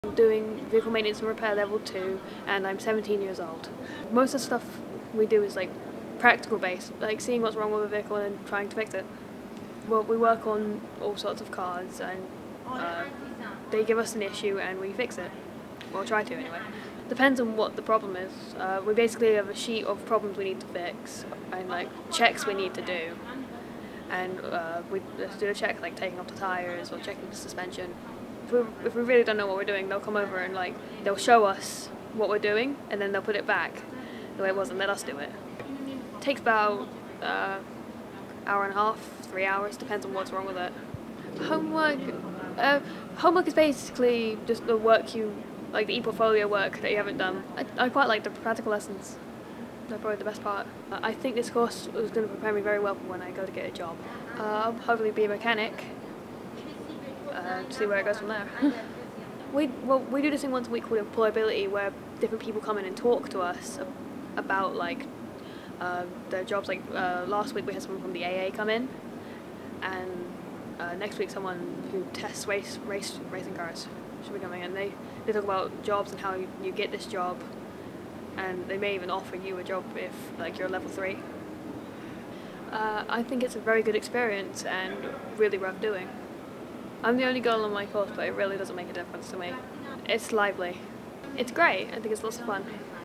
Level 2 Vehicle Maintenance & Repair
Barnet and Southgate College Vehicle Maintenance and Repair student Jade talks about her course - the workload, the nature of assignments, support from tutors and what she likes best about her programme of study. Follow the link to see more course details